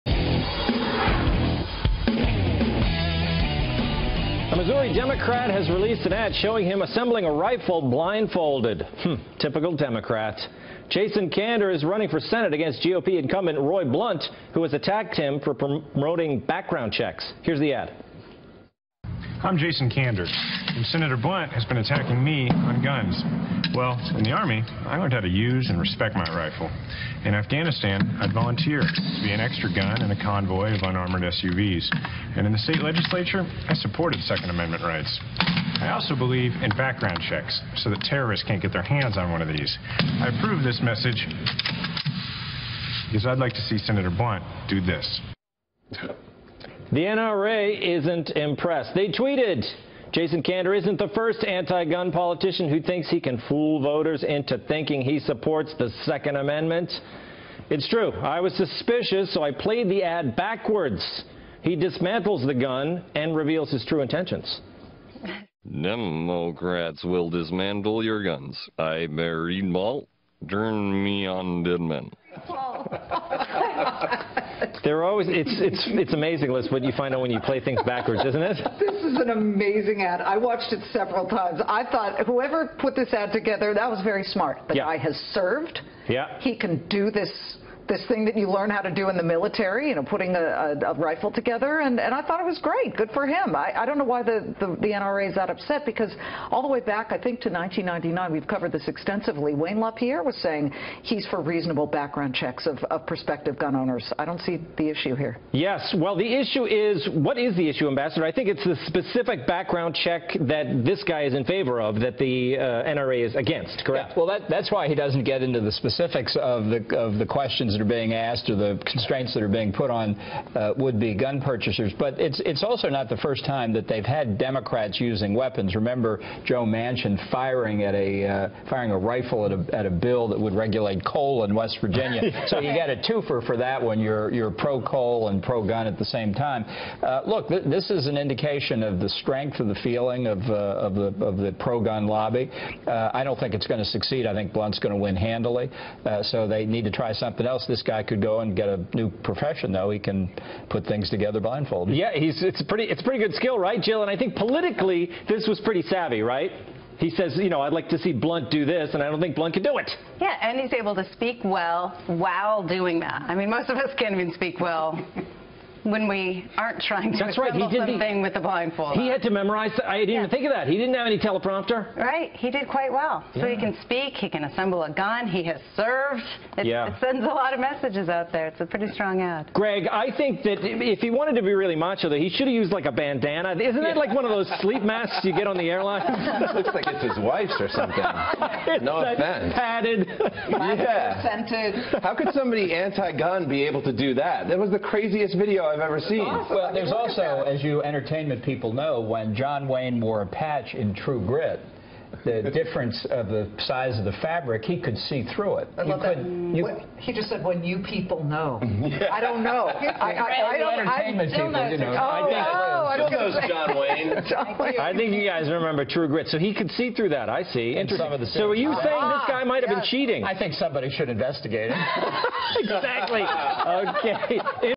Democrat assembles rifle while blindfolded in ad
Missouri Senate candidate Jason Kander takes heat from NRA